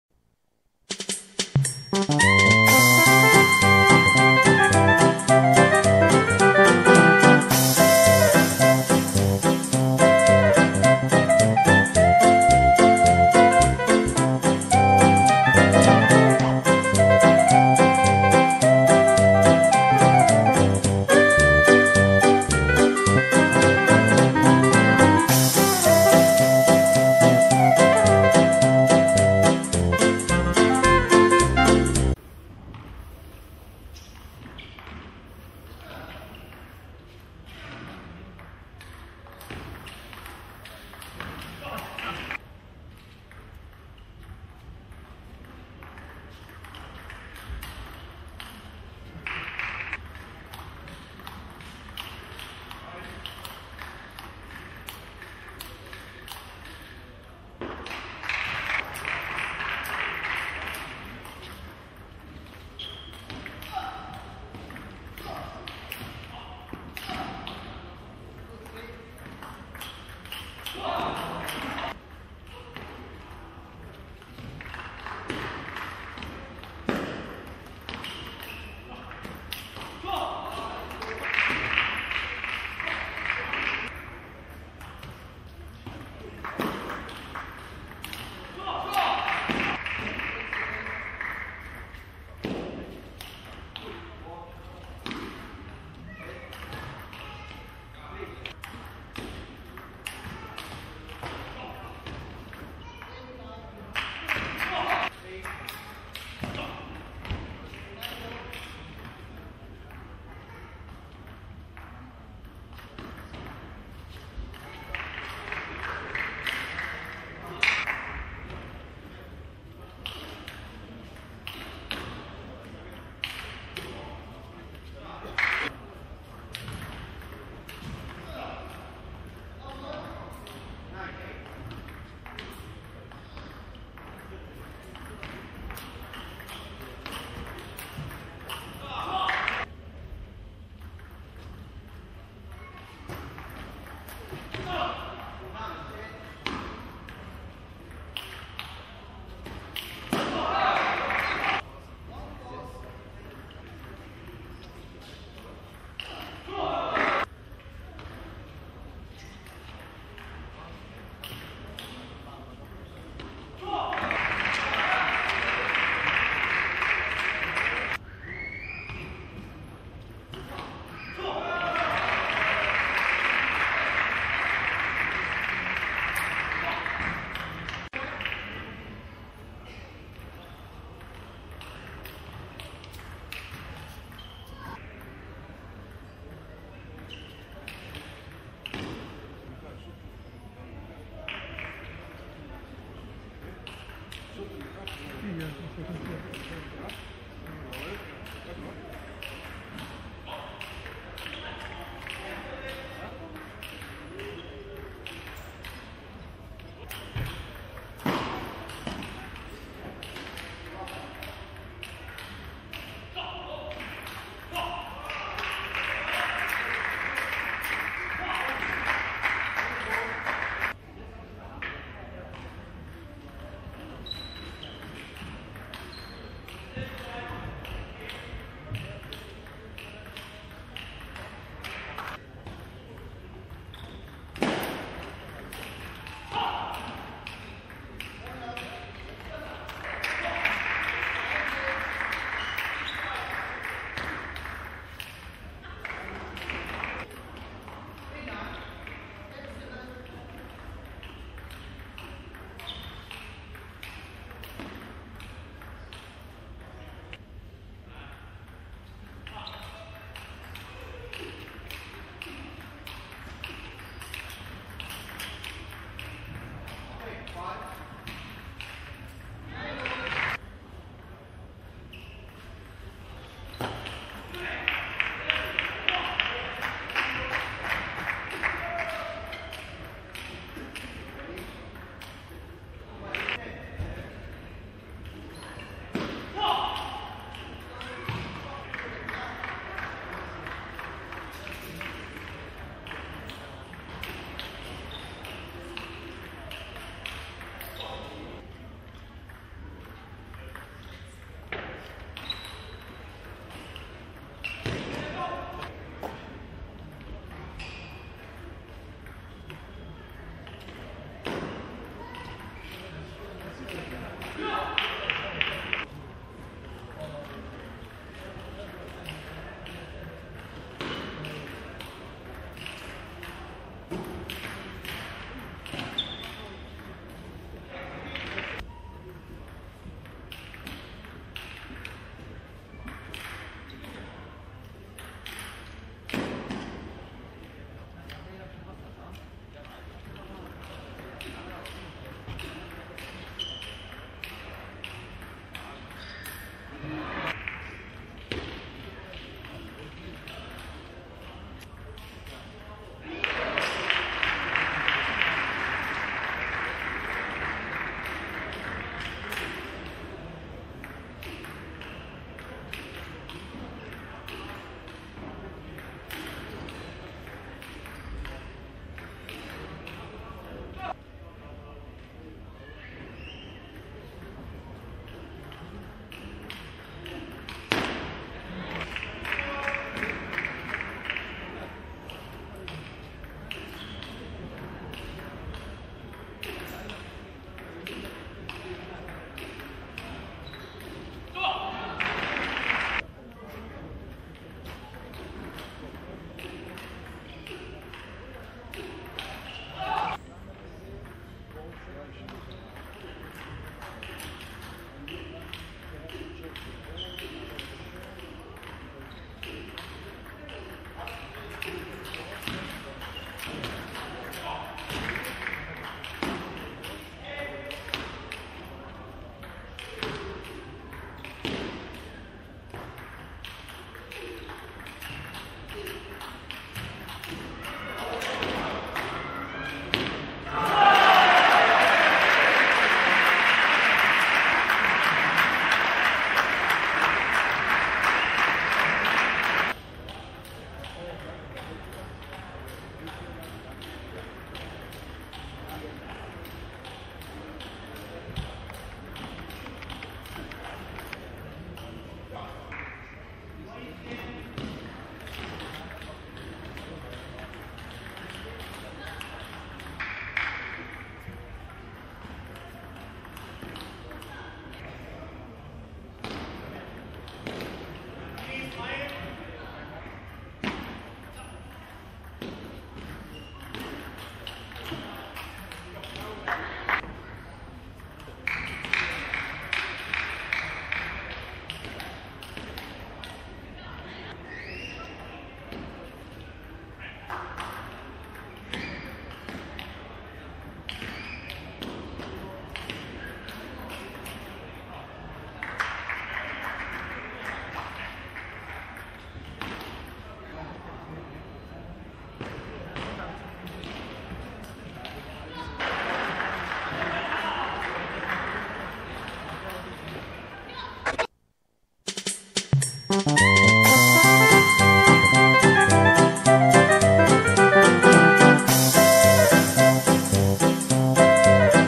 2009 Qatar Open part 7
2009 Qatar Open part 7. Timo boll in full swing. Great Video. Watch out for part 8